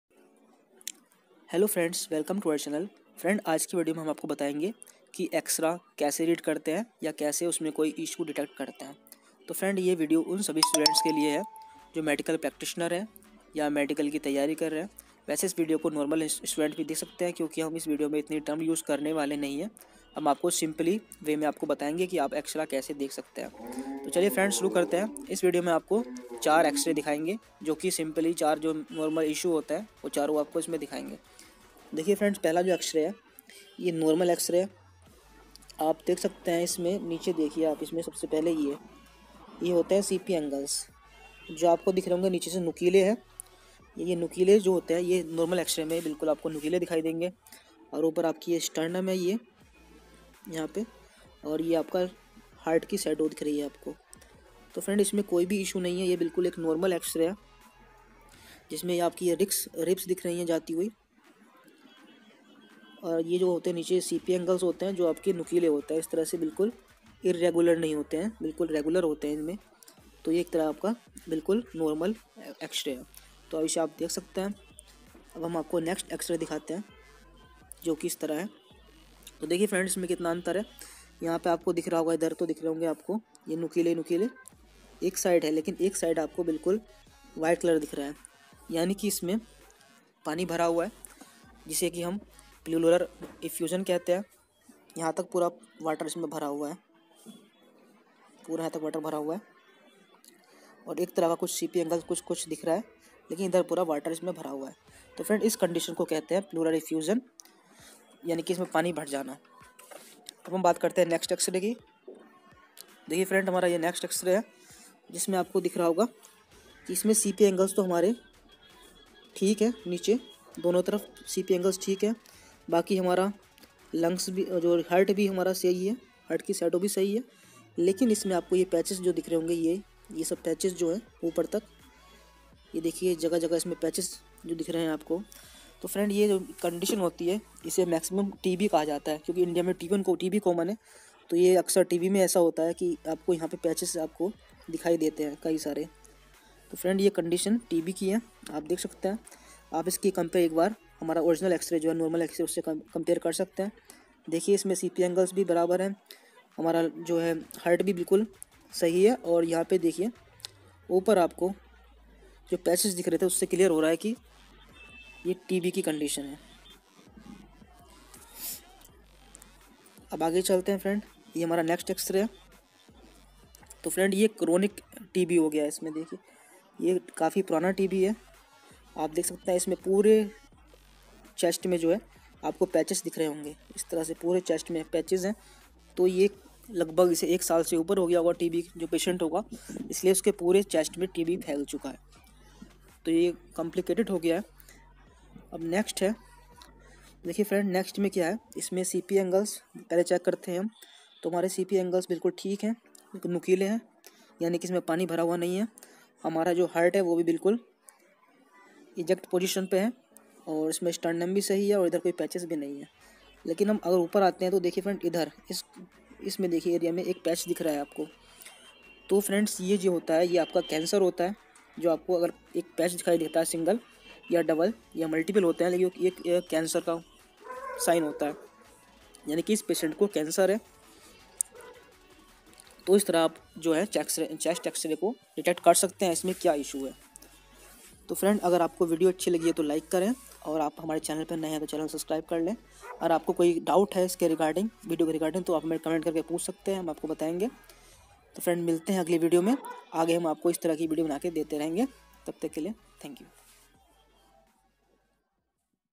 How to read Chest X-ray || Hindi language
Hello friends

in this video we show you how to read X-ray.
Firstly we talk about Chest X-ray.
there are four major complications in chest .

1- Pleural Effusion
2- Heart Failure
3- TB
4- Chest Cancer